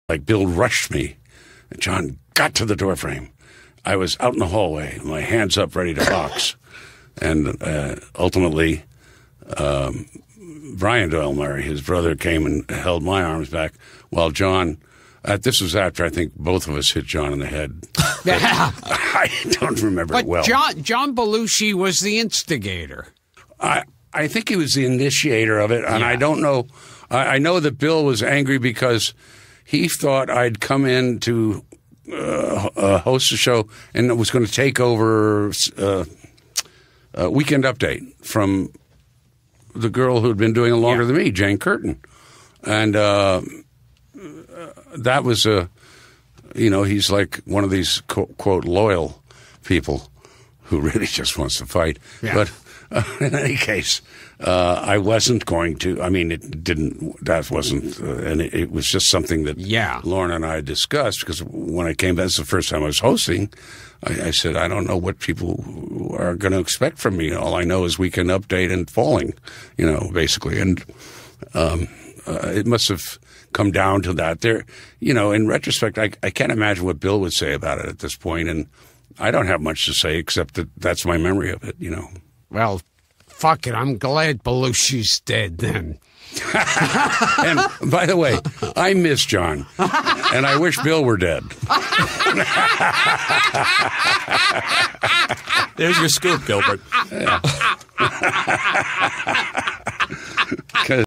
Chevy Chase recalls his fight with Bill Murray
Chevy  tells Gilbert and Frank about his fight withBill at SNL.